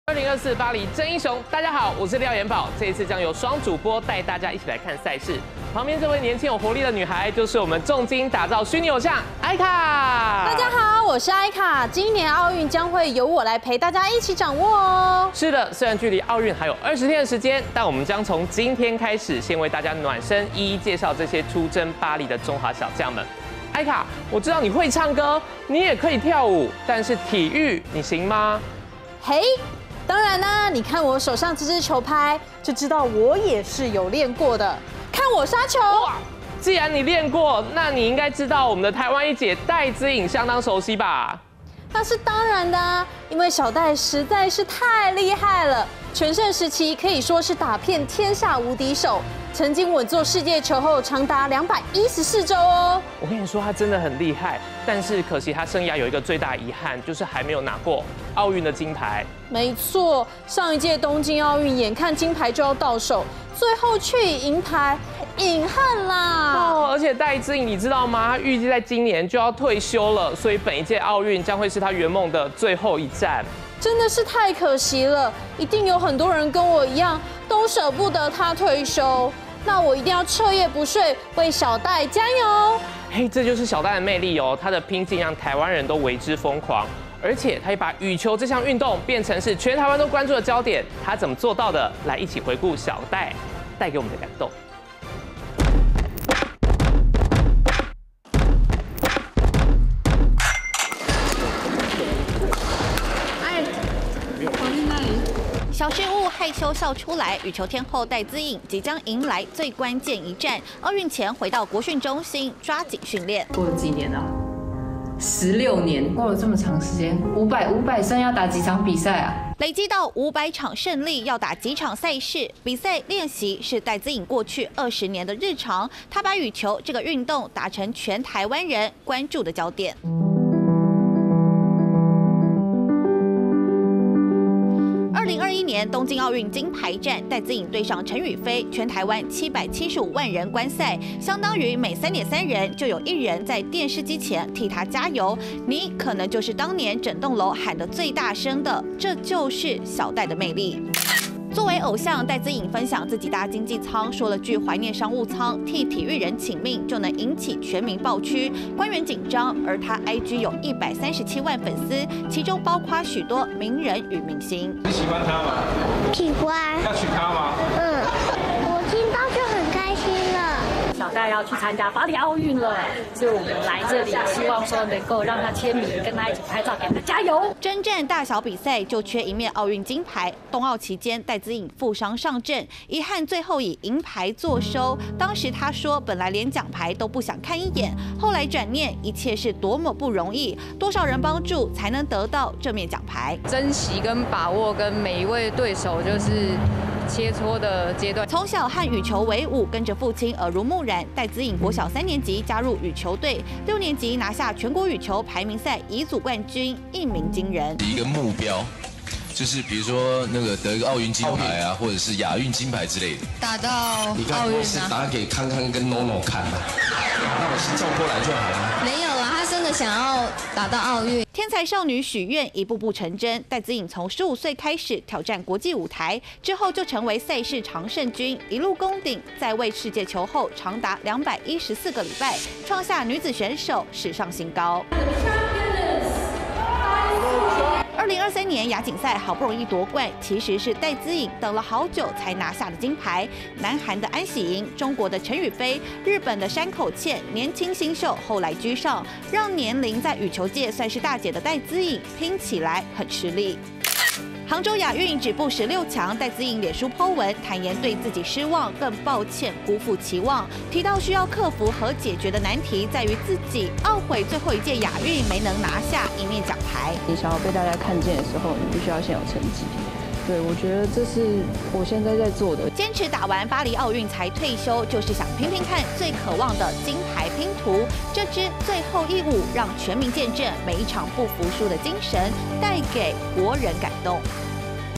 戴資穎 前進巴黎最後一舞 傳奇球后力拚金牌｜三立娛樂星聞
★訂閱【女孩要幹嘛】甜寵浪漫戲劇看到飽